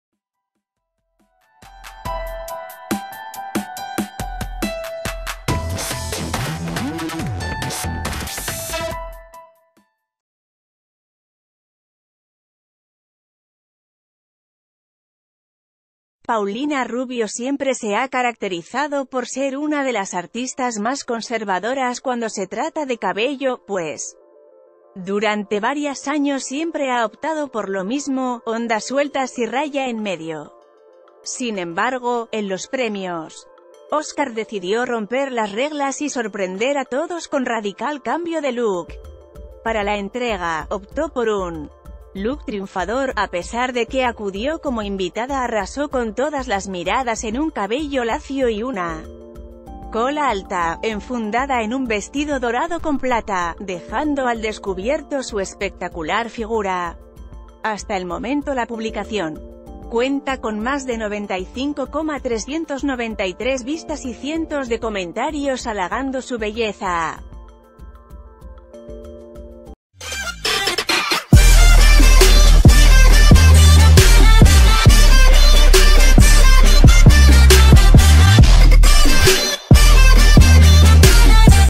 Paulina Rubio sorprende con radical cambio de look
Paulina Rubio siempre se ha caracterizado por ser una de las artistas más conservadoras cuando se trata de cabello, pues durante varias años siempre ha optado por lo mismo, ondas sueltas y raya en medio. Sin embargo, en los Premios Oscar decidió romper las reglas y sorprender a todos con radical cam...